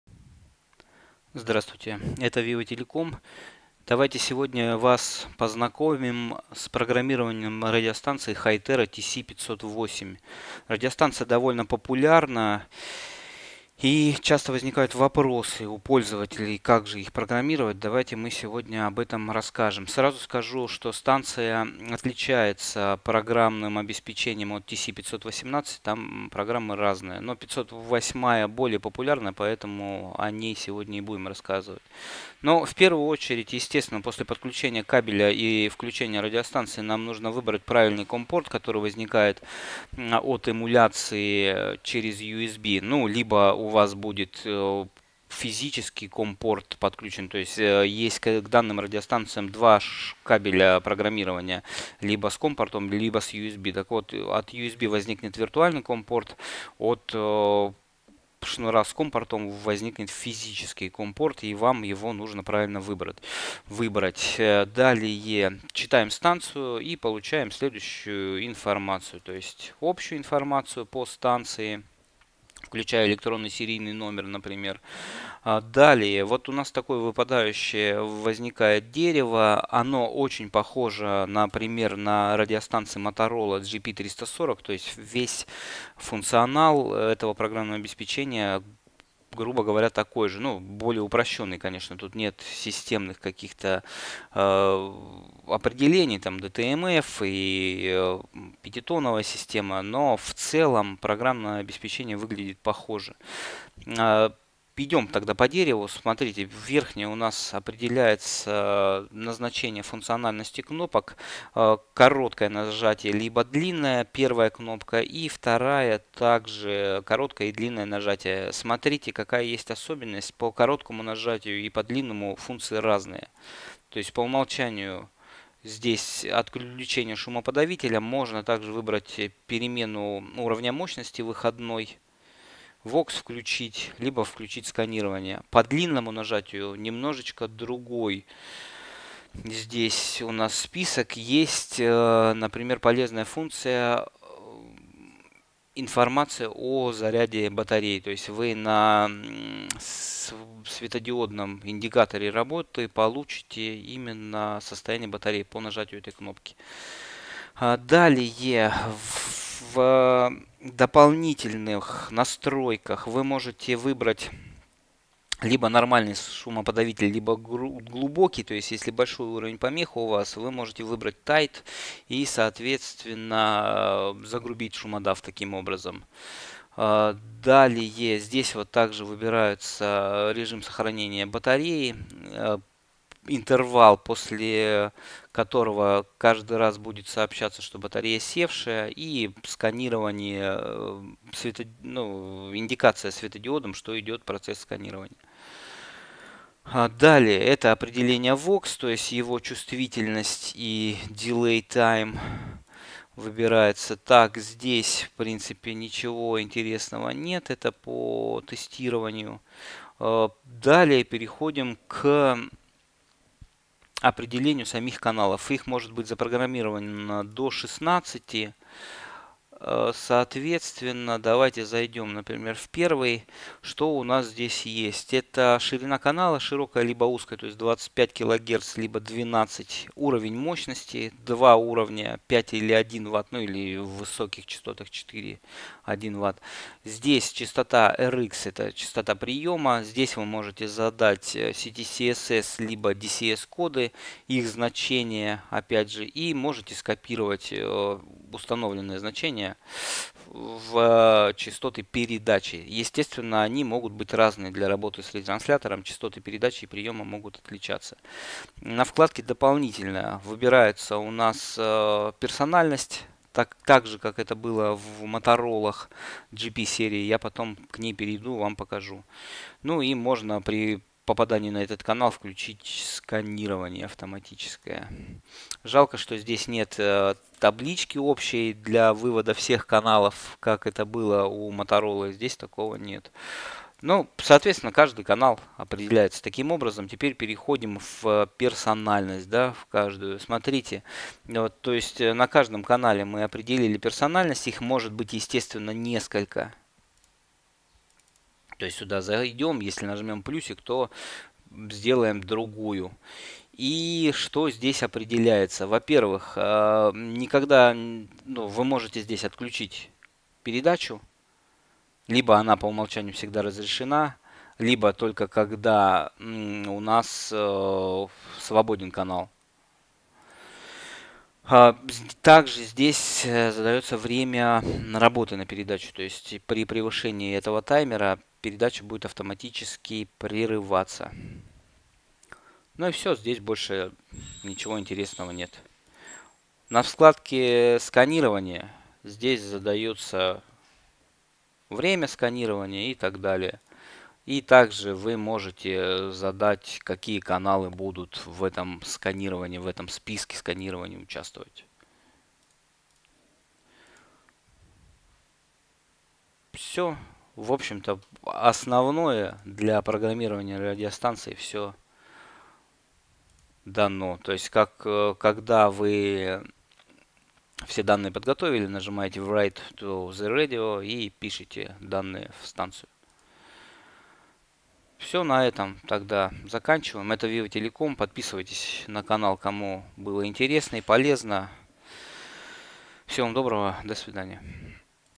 Программирование радиостанции Hytera TC-508
Обучающий ролик по программированию радиостанции Hytera TC-508.
Назначение кнопок, программирование каналов и поднесущих.

Подробные технические характеристики, наличие и стоимость радиостанций Hytera TC-508 смотрите на сайте Вива-Телеком: